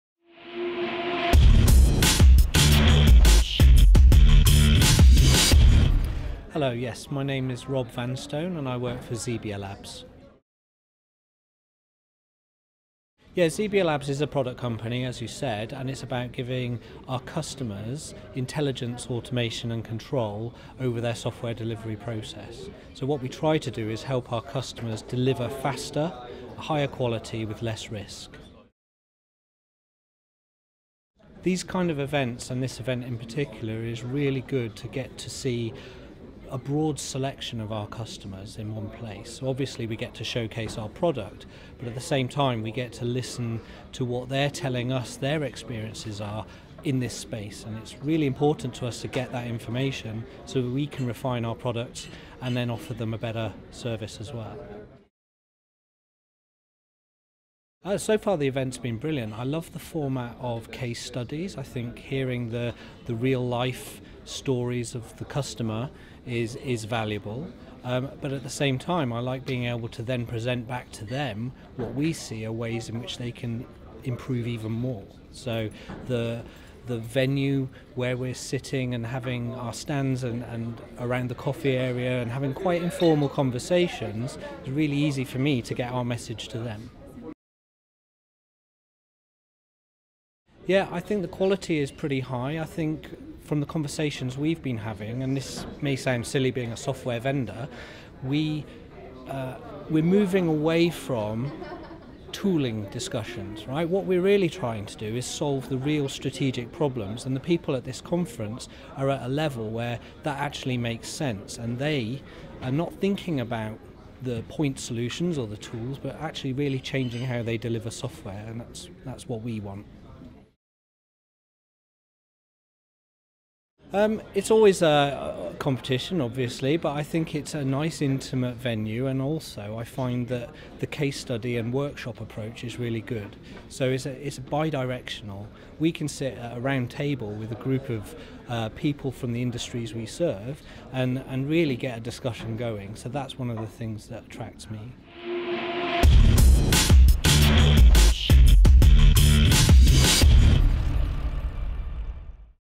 fin:CODE Testimonial 2018 - XebiaLabs
We had a chat with one of our Business Partners Brent Beer from  GitHub at the fin:CODE in London on his impressions of the event.